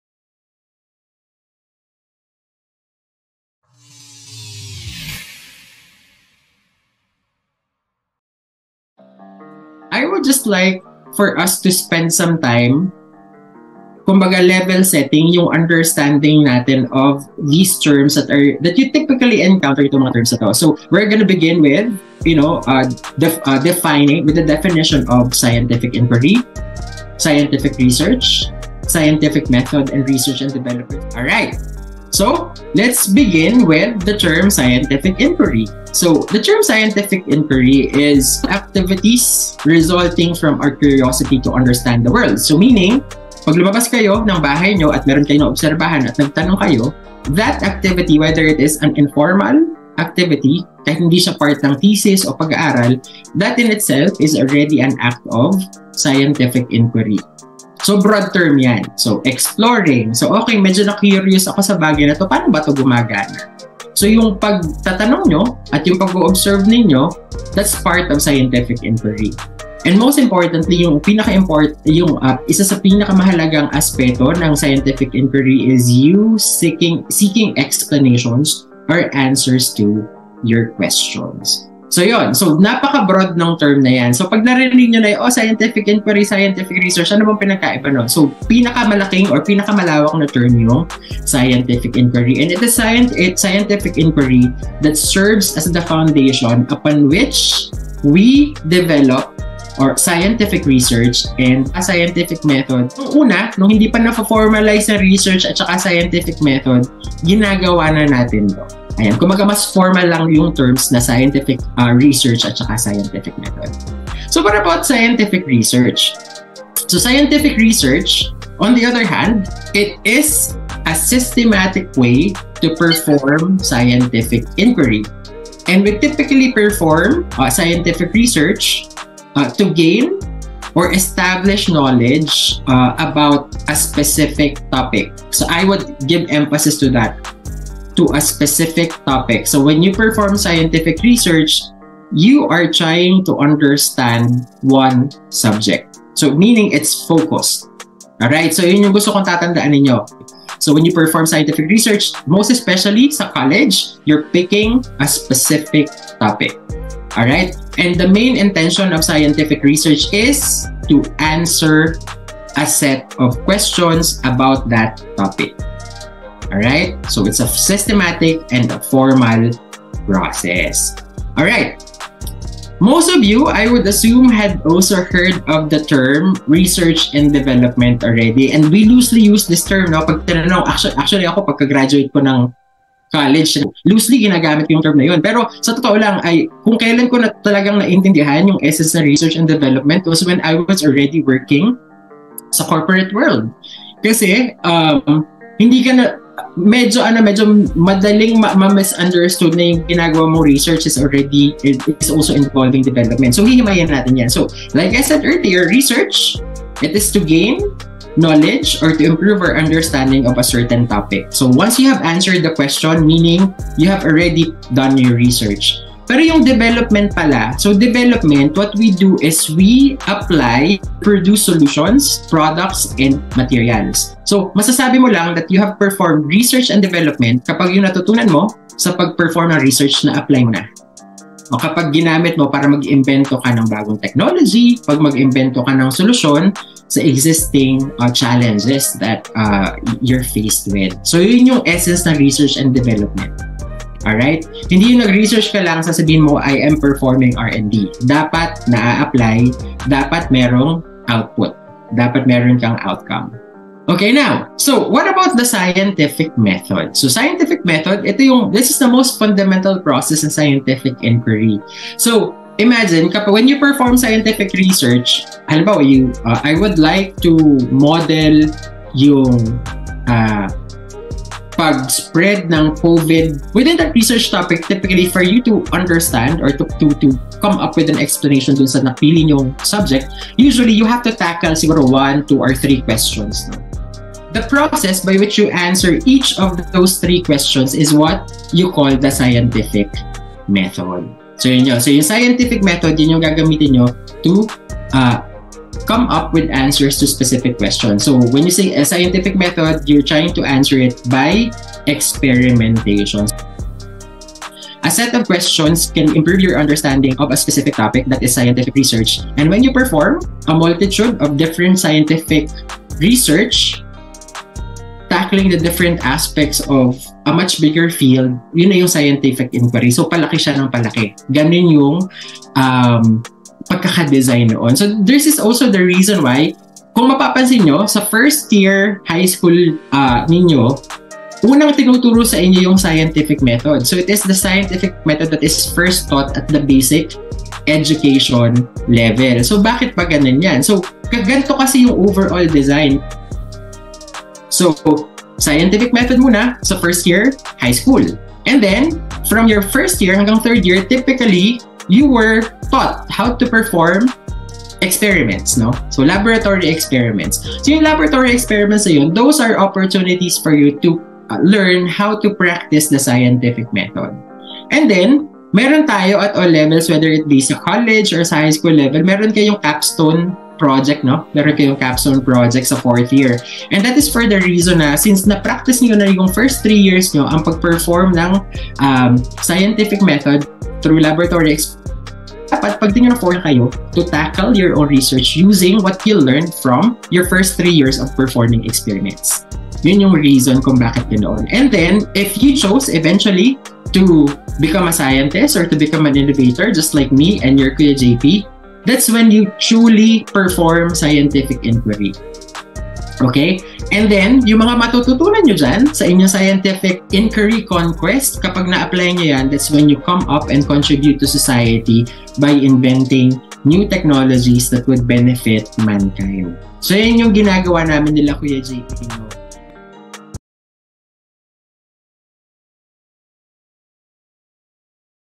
RESEARCH BASICS: Definition of Terms
-- Scientific Inquiry
-- Scientific Research
-- Scientific Method
-- Research & Development
featuring FilSciHub Founder, Jeffrey Camacho-Bunquin